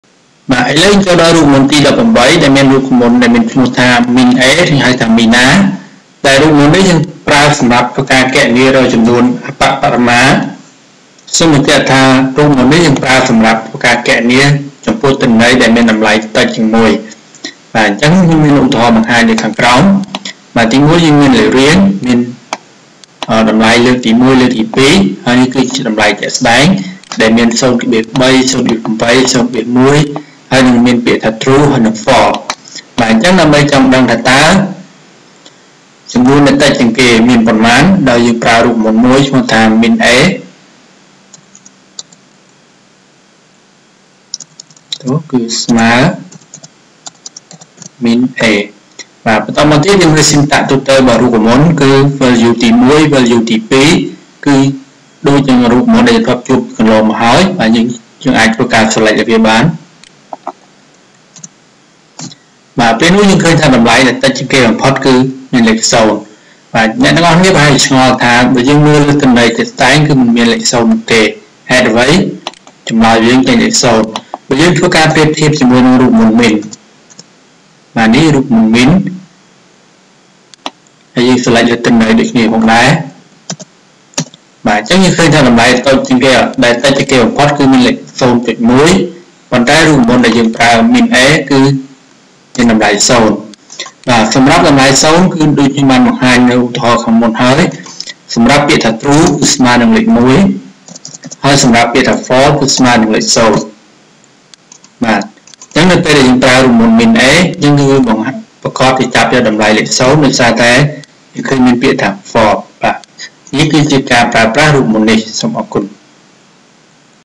18 MINA
រូបមន្ត MINA ប្រើសម្រាប់ធ្វើការគណនារកចំនួនដែលតិចជាងគេបំផុត (សម្រាប់ចំនួនតិចជាង១ ដោយមានលាយអក្សរ គឺ True False ដោយ True=1និង False =0)។